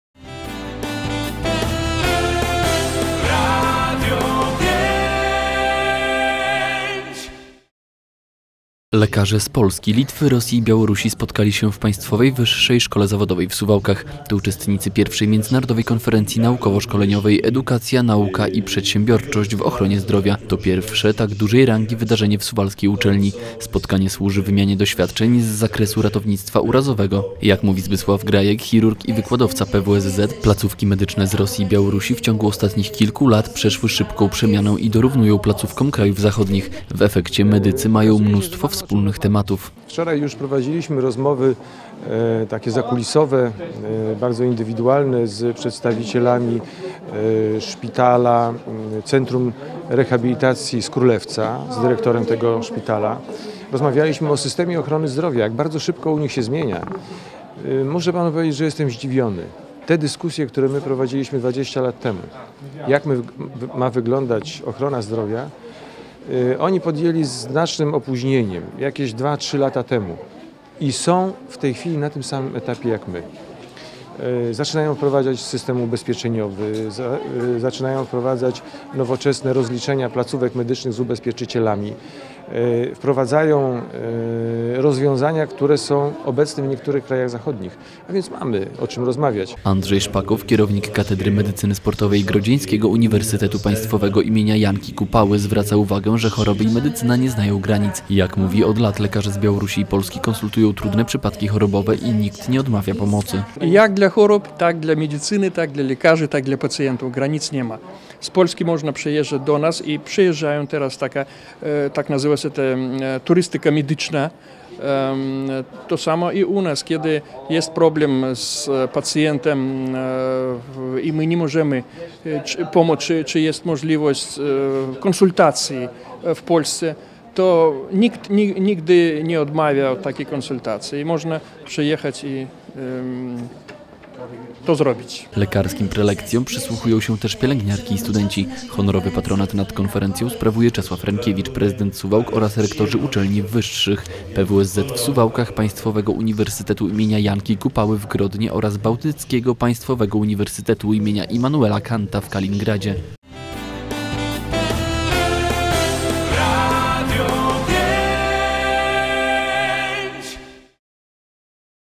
Medycyna nie zna granic
Lekarze z Polski, Litwy, Rosji i Białorusi spotkali się  w Państwowej Wyższej Szkole Zawodowej w Suwałkach. To uczestnicy I Międzynarodowej Konferencji Naukowo-szkoleniowej " - Edukacja, nauka i przedsiębiorczość w ochronie zdrowia". To pierwsze tak dużej rangi wydarzenie w suwalskiej uczelni. Spotkanie służy wymianie doświadczeń z zakresu ratownictwa urazowego. Jak mówi  Zbysław Grajek, chirurg i wykładowca PWSZ, placówki medyczne z Rosji i Białorusi w ciągu ostatnich kilku kilku lat przeszły szybką przemianę i dorównują placówkom krajów zachodnich. W efekcie medycy mają mnóstwo wspólnych tematów. Andrzej Szpakow, kierownik katedry medycyny sportowej Grodzieńskiego Uniwersytetu Państwowego im. Janki Kupały zwraca uwagę, że choroby i medycyna nie znają granic.  Jak mówi, od lat lekarze z Białorusi i Polski konsultują trudne przypadki chorobowe i nikt nie odmawia pomocy. Lekarskim prelekcjom przysłuchują się tez pielęgniarki i studenci. Honorowy patronat nad Konferencją sprawuje Czesław Renkiewicz - prezydent Suwałk oraz rektorzy uczelni wyższych: PWSZ w Suwałkach, Państwowego Uniwersytetu im. Janki Kupały w Grodnie oraz Bałtyckiego Państwowego Uniwersytetu im. Immanuela Kanta w Kaliningradzie.